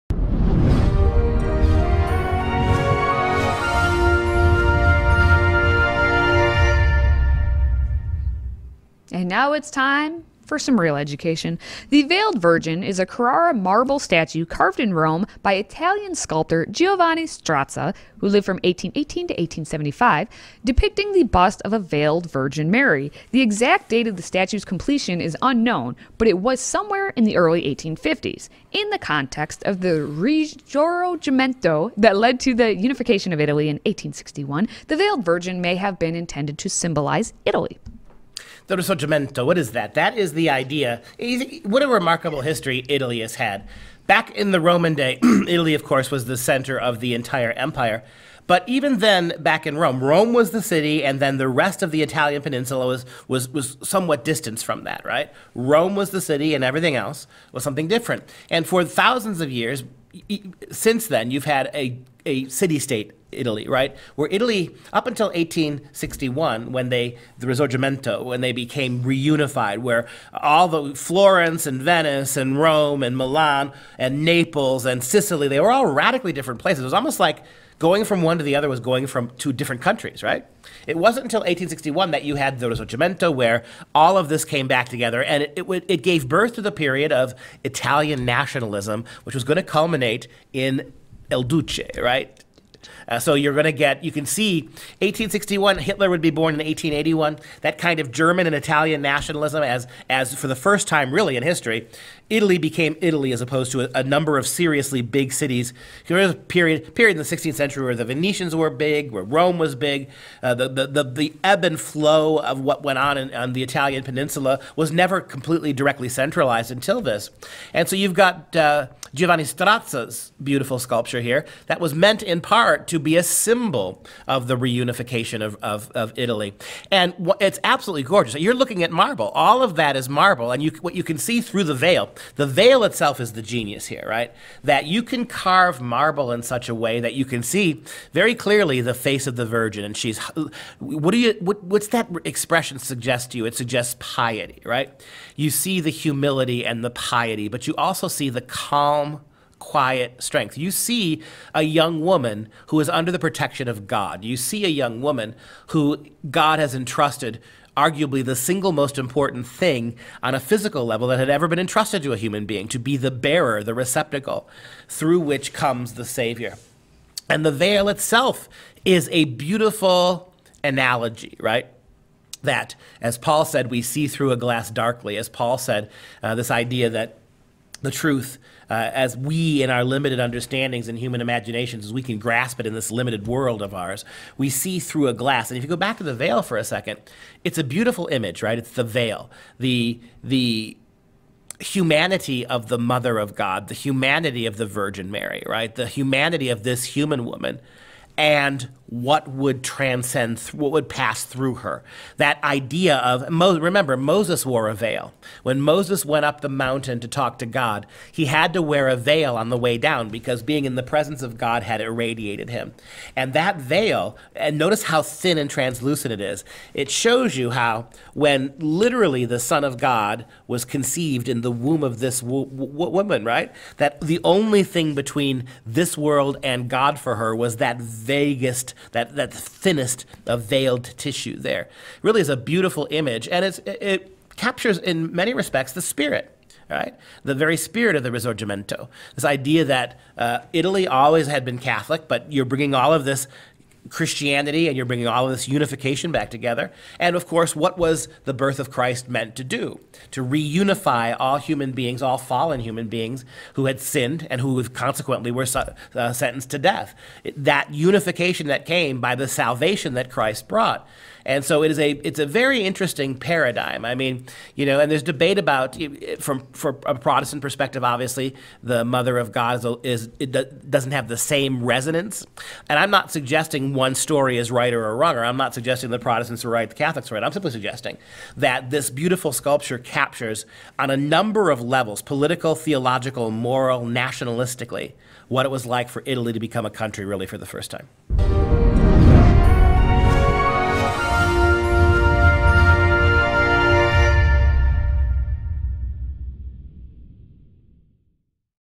The Veiled Virgin
Dr. Duke and Katie discuss 'The Veiled Virgin,' and why it is truly an Instant Classic.